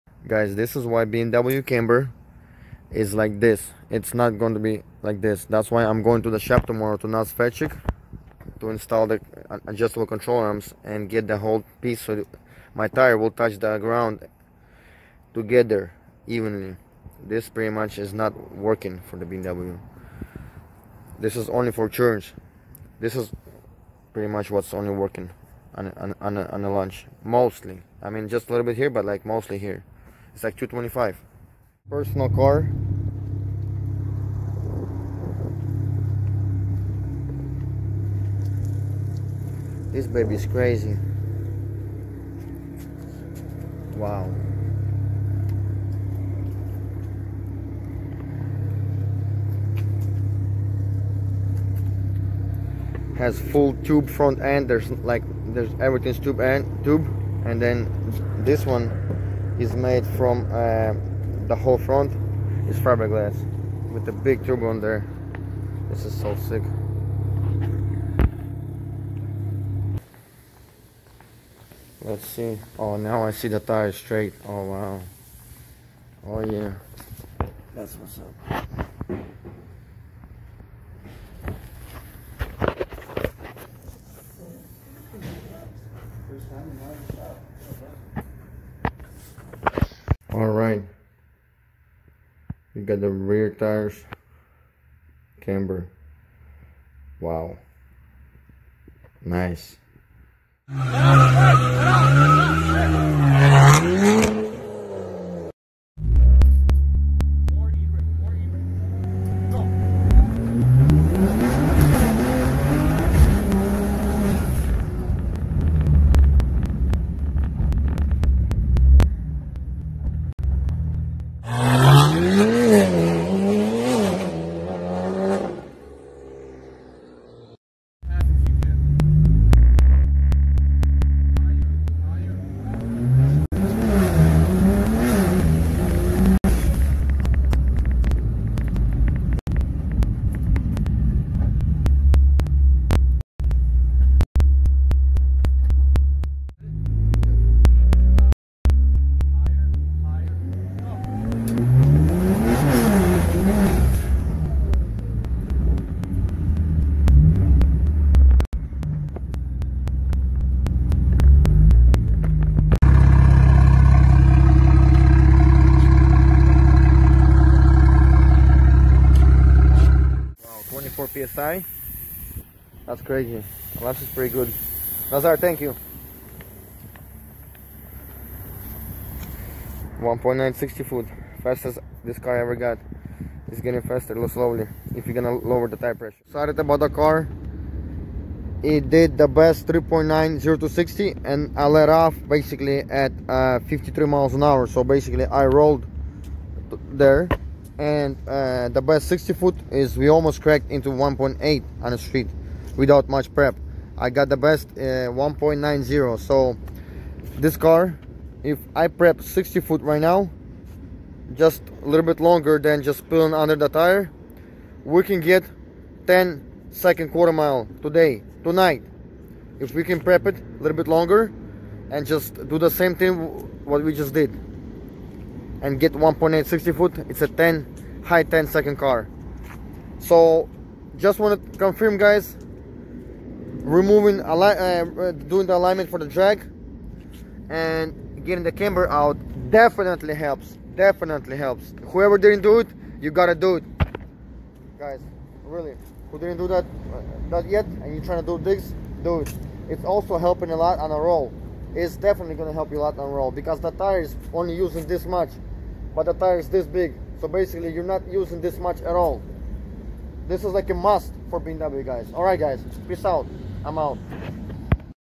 removing camber for more grip on 335 and trying out 60ft!
finally guys finally the car is running amazing!! I dont think I ever had this car running this perfect! no missfires nothing! super smooth! personal best on 60 ft (1.90 !!) on the street with just little prep and no practice! if you want your bmw not to have only 195 tire size that grips and want all of the 275 or 285 then get adjustable control arms and do custom drag alignment! helps so much! my Instagram alex_glassreflections

I dont get payed for it just wanted to show you guys where you can get some yourselfers